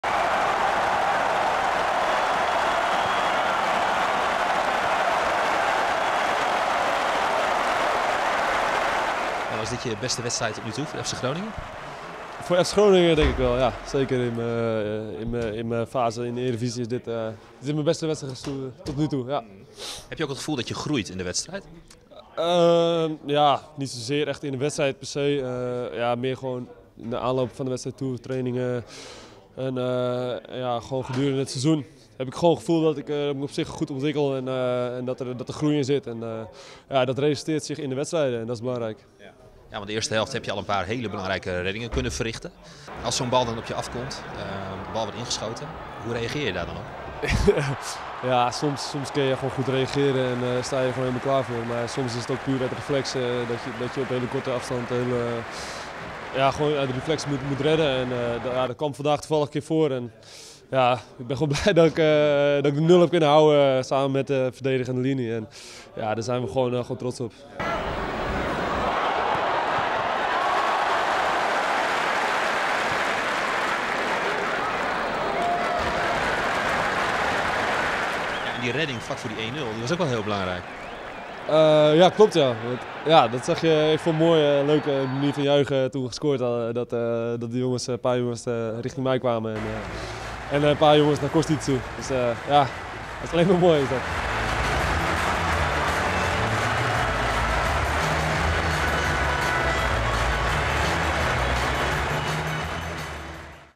Marco Bizot: 'Dit was mijn beste wedstrijd in de eredivisie'
Essent Man of the Match Marco Bizot na afloop van FC Groningen - PSV in gesprek met Emiel Venema van FC Groningen-TV.